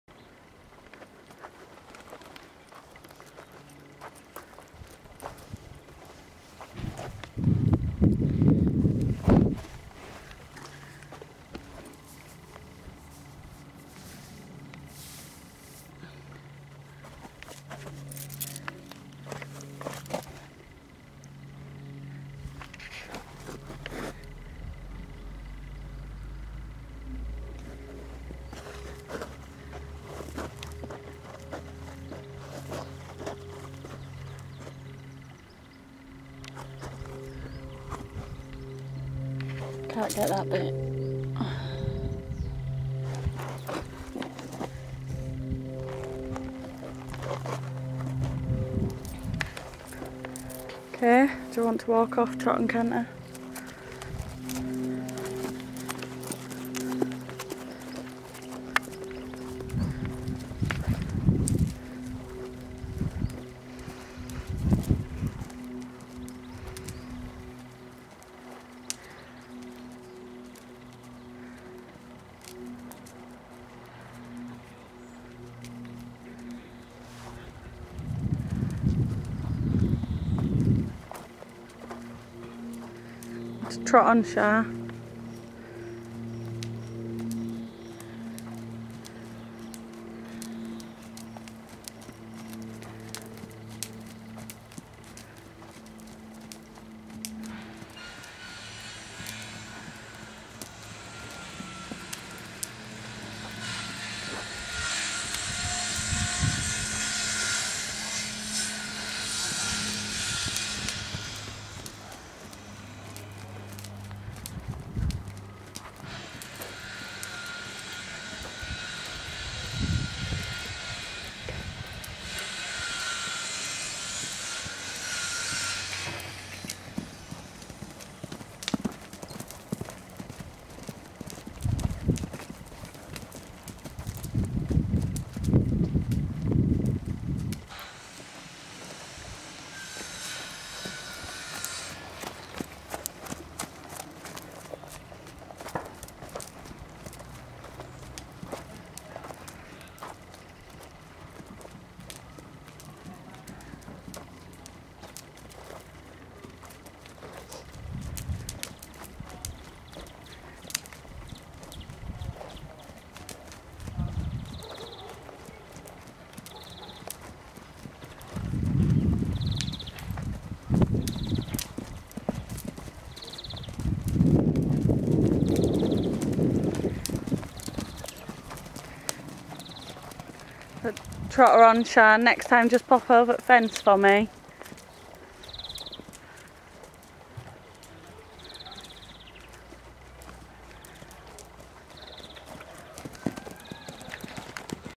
Goldie School
For Sale 13.2hh, 13 yr old dun mare, school work. Harlow , Essex.